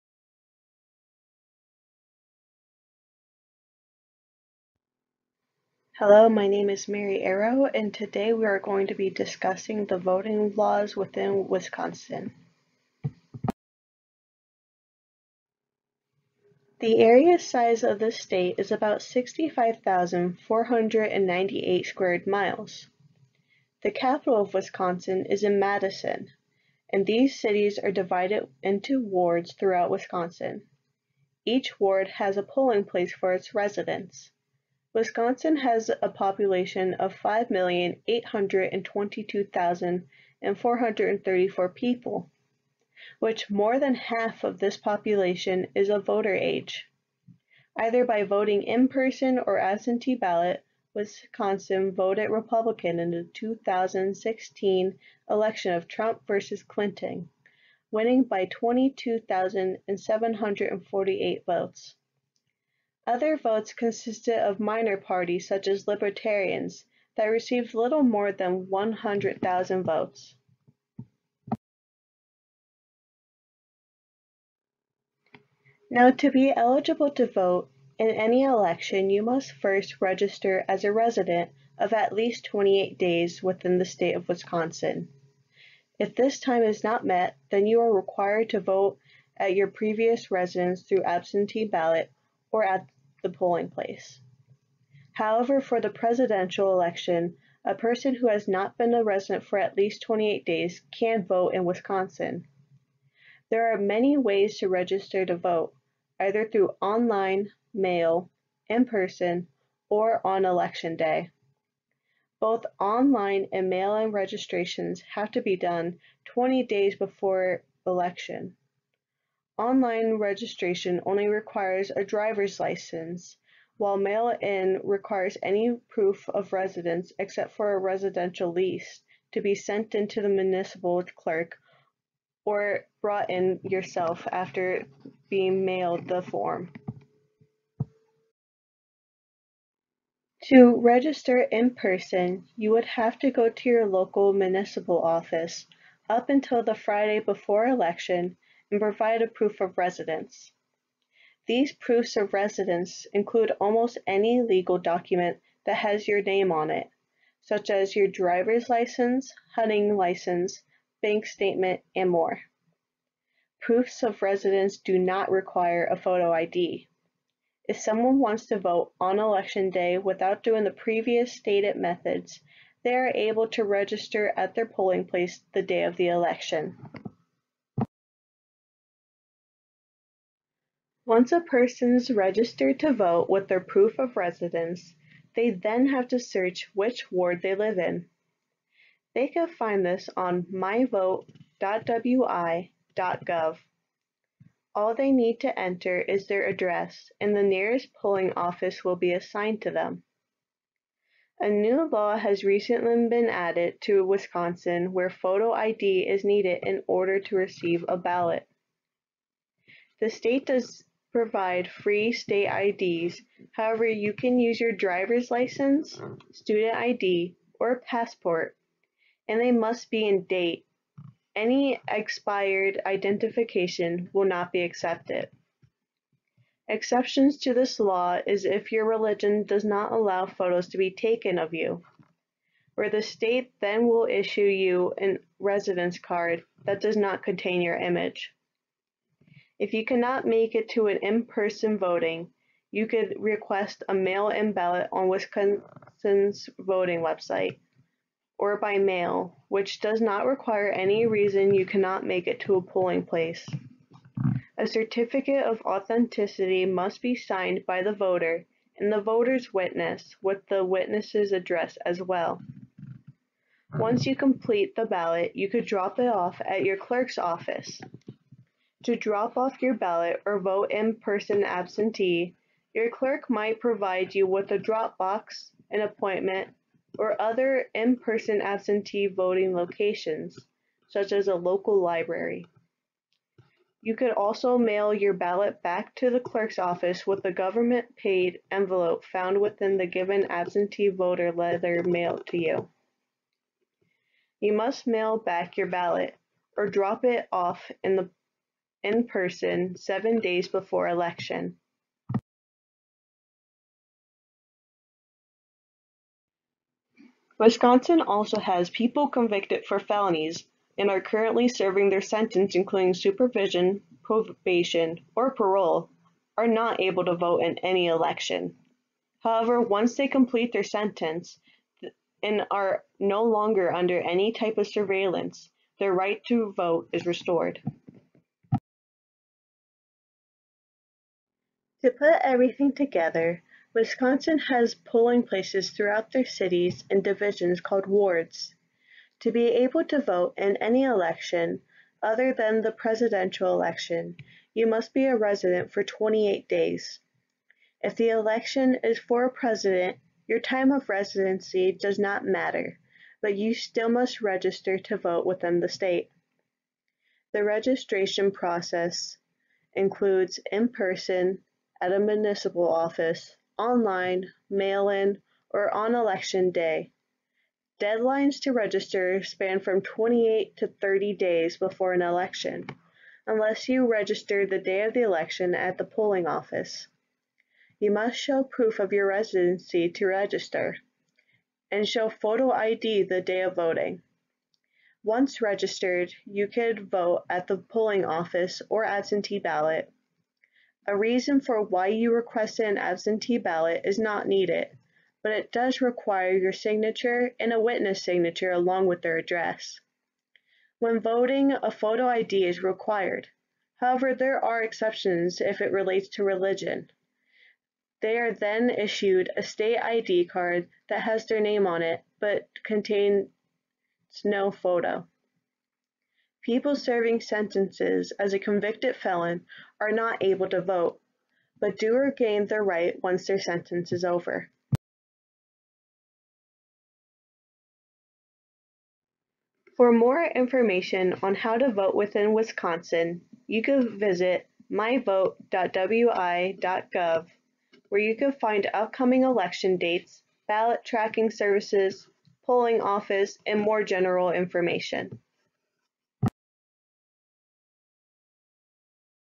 How to Vote in Wisconsin
Learn how to register and vote, and some other voting laws within Wisconsin.